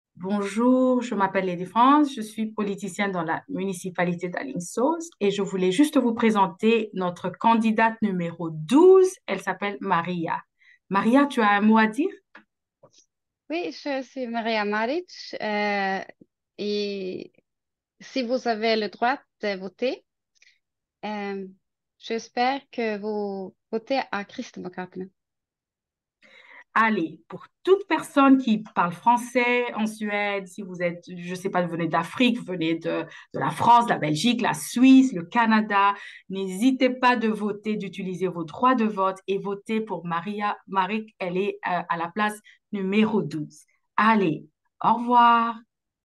Utilisez votre droit de vote aux élections du Parlement européen!
Si vous vivez en Suède, n’hésitez pas à voter pour les chrétiens-démocrates (KD) et pour Maria Maric!